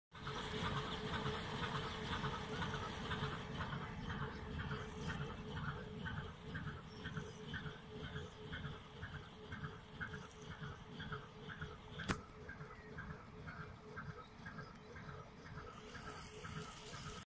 Mooie oude sleepboot.
Sleepboot in onberispelijke staat kwam langs varen op de Maas.